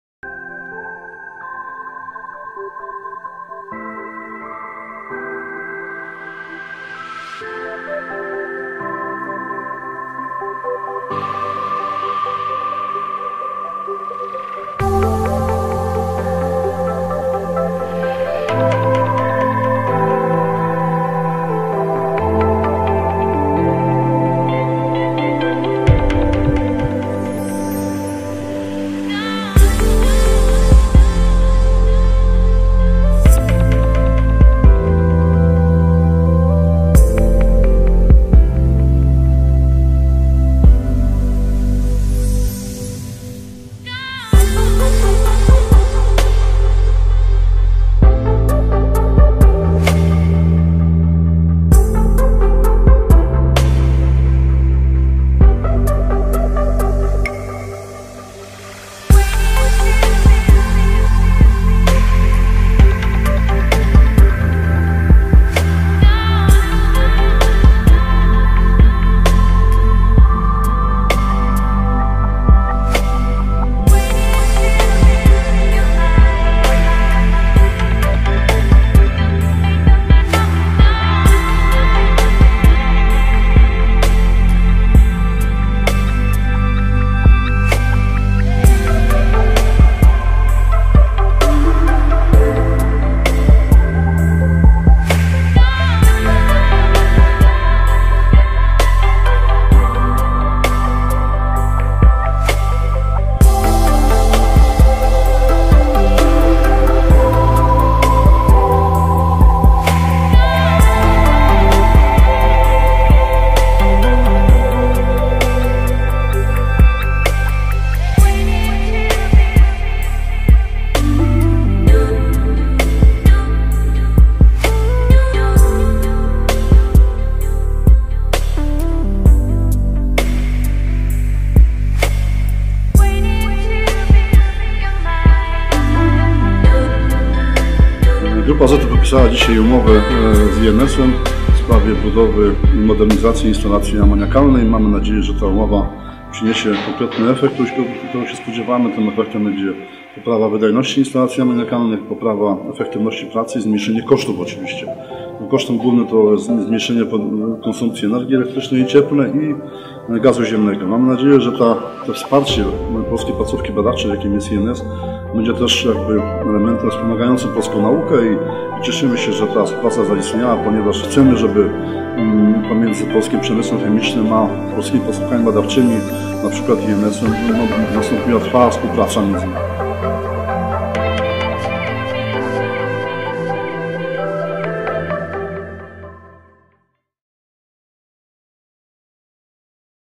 Grupa Azoty Police zmodernizuje instalacje do produkcji amoniaku!
Dzięki podpisanej 10 grudnia w Warszawie, wartej 46 mln PLN umowie z INS, spółka w najbliższych latach zwiększy i zoptymalizuje jego produkcję. Jest to odpowiedź Grupy Azoty na światowy deficyt amoniaku oraz wciąż rosnące na rynkach ceny. Dodatkowo projekt dowodzi, że współpraca z polską nauką może przekładać się na zyski przedsiębiorstw

Do sfinalizowania porozumienia „Modernizacja procesu syntezy amoniaku w Grupie Azoty Zakłady Chemiczne „Police” S.A.” doszło przy okazji odbywającej się w Warszawie konferencji Nafta/Chemia. Przy złożeniu podpisu obecni byli Minister Skarbu Państwa Włodzimierz Karpiński, Prezes Zarządu Grupa Azoty S.A. Paweł Jarczewski, Prezes Zarządu Grupy Azoty Police Krzysztof Jałosiński oraz Dyrektor Instytutu Nowych Syntez Chemicznych (INS) Cezary Możeński